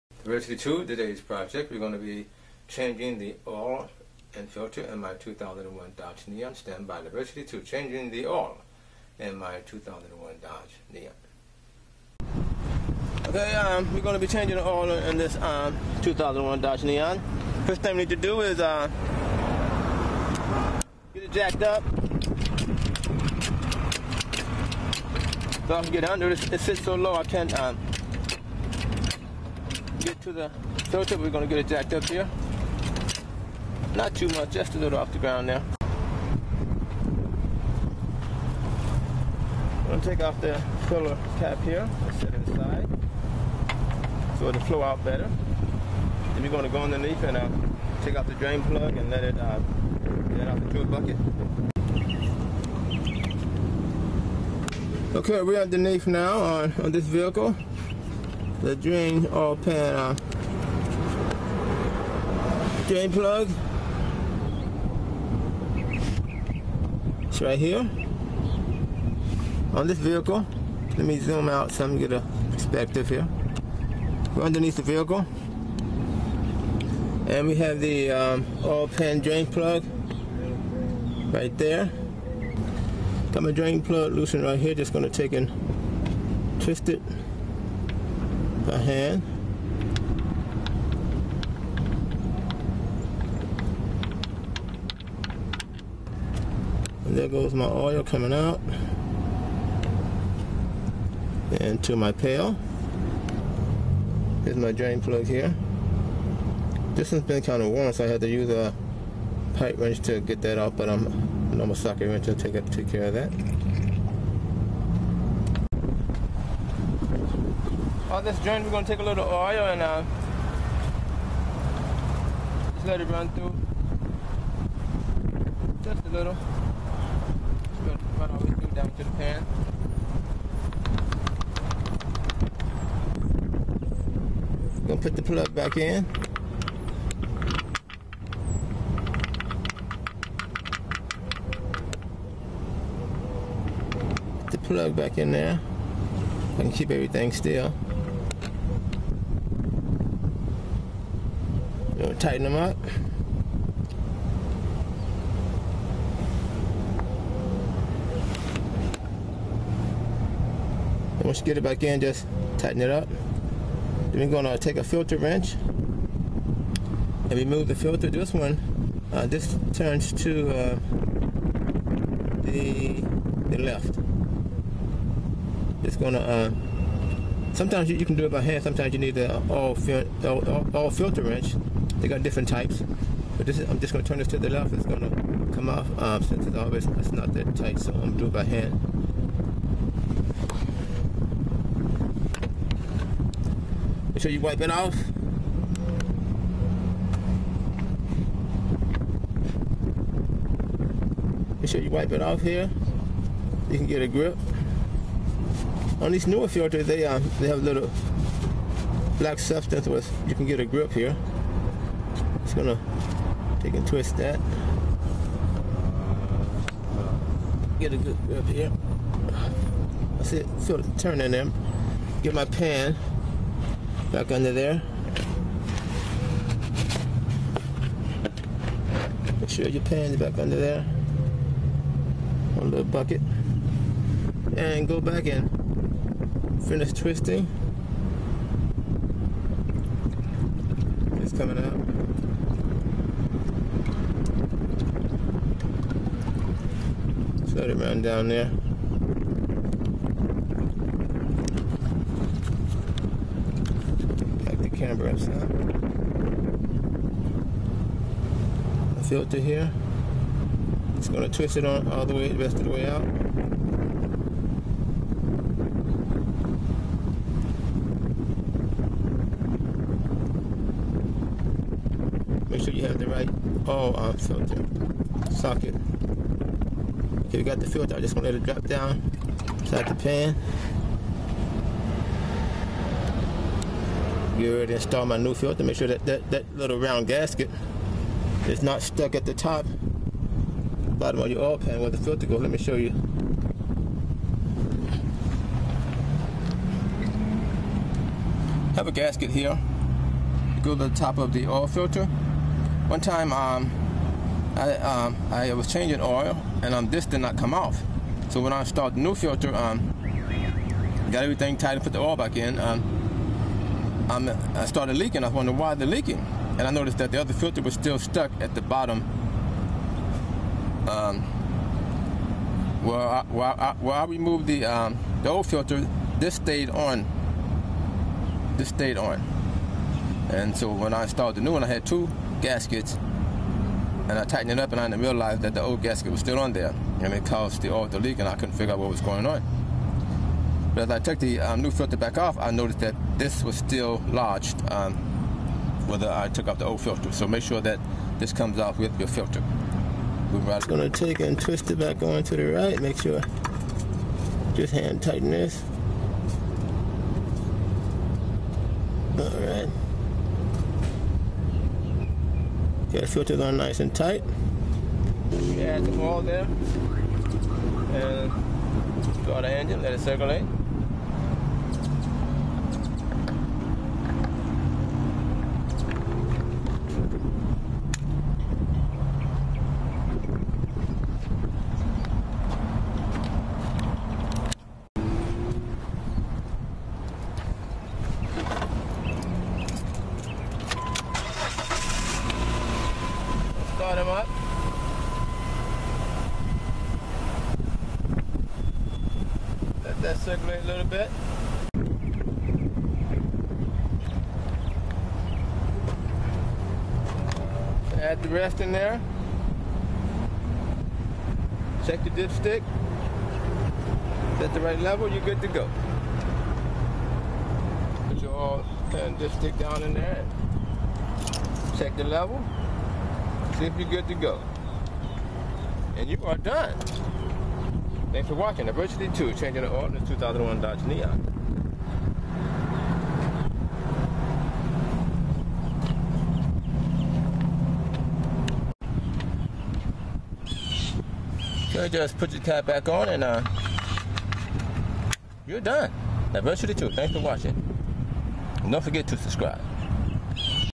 How to change oil in a 2001 Dodge neon and Plymouth neon
In this viedo I am showing how I change my oil in my 2001 Dodge neon